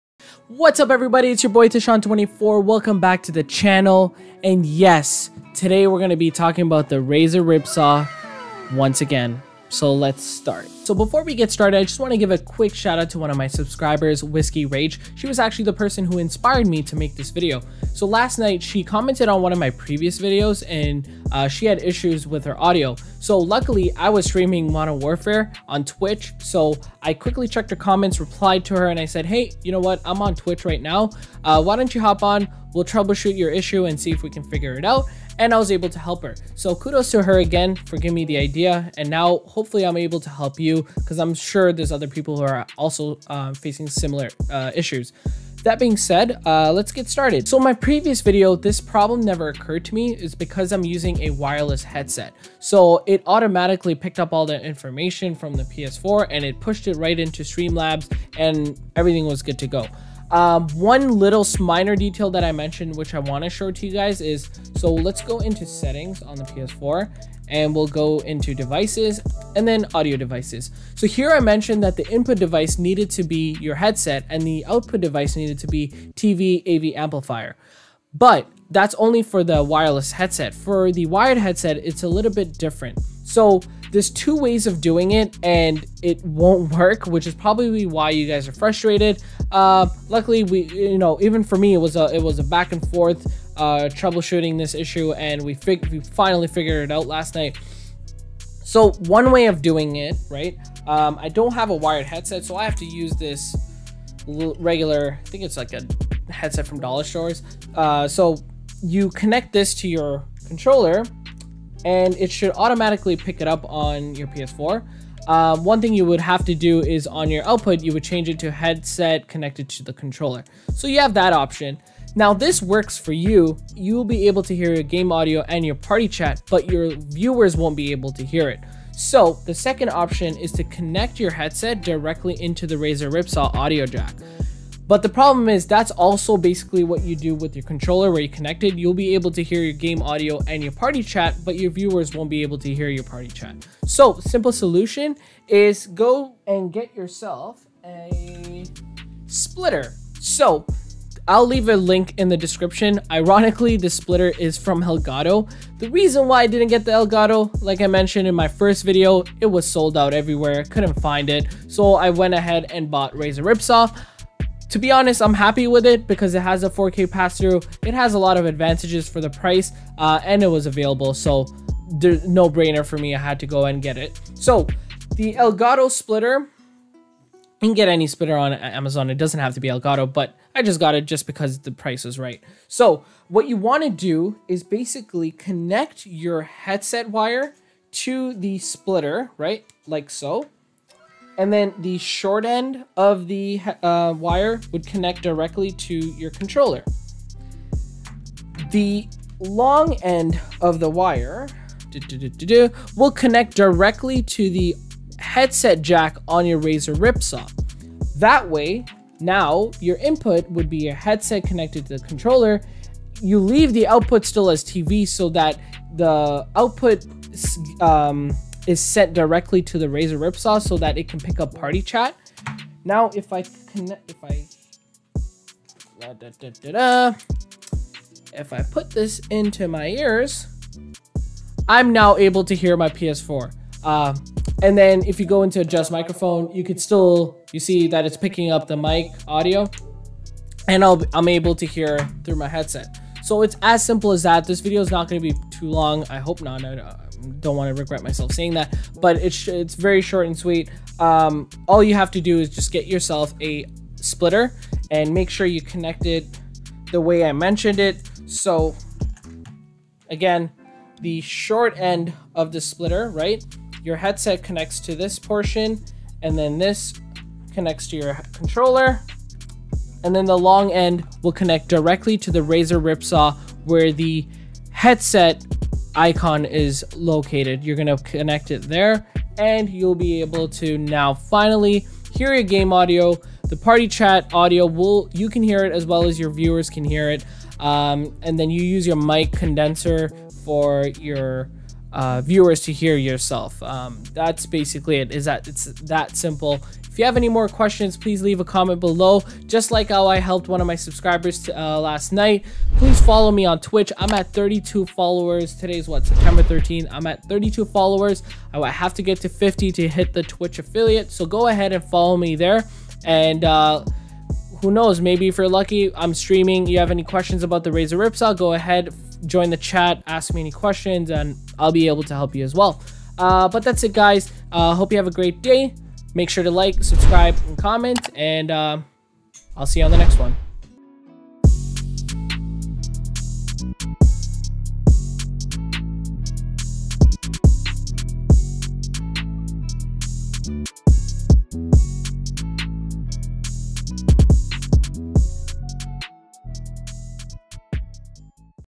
How to setup: PS4 game and party chat audio using the Razer Ripsaw HD
One of my subscribers commented on my previous video looking for answers. I made a quick tutorial for streamers to get their PS4 game and party chat audio to work using the Razer Ripsaw HD.

Please watch my previous series regarding the Razer Ripsaw

Part 1:  (How To Setup: Razer Ripsaw HD (PS4 or Xbox)

Part 2:(The perfect audio setup for your PS4 streaming using Razer Ripsaw HD and StreamLabs (OBS)

As mentioned in the video here's a link to Elgato splitter:

11nferno Beats